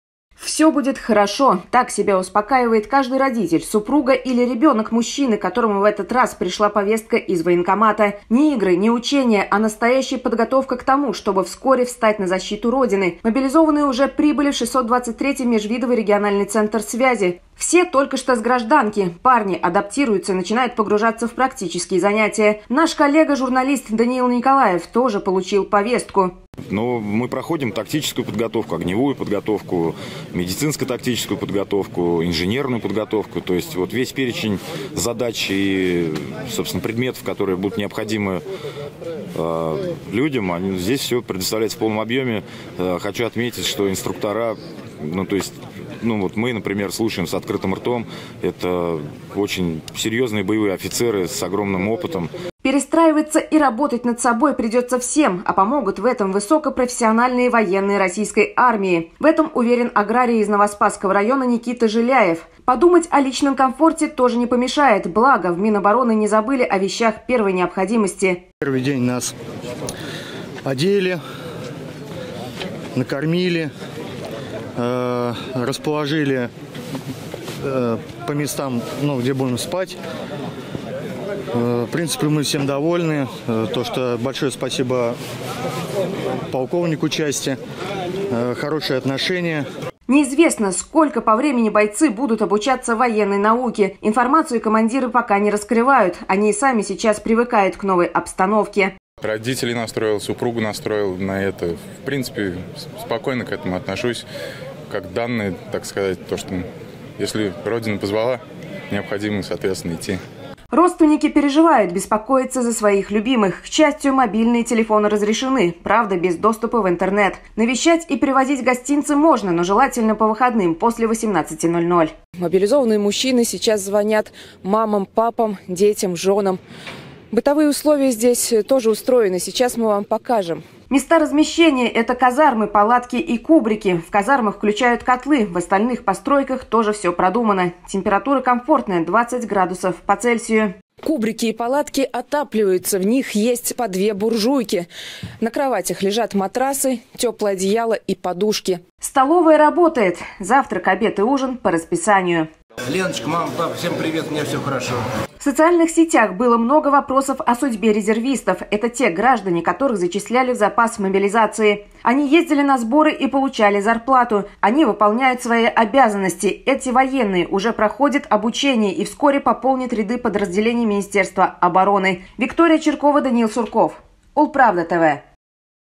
«Если Родина позвала». В каких условиях живут мобилизованные в Ульяновске
Верховный главнокомандующий Российской Федерации объявил частичную мобилизацию. Наша съемочная группа отправилась в учебный центр и посмотрела, как там живут мобилизованные.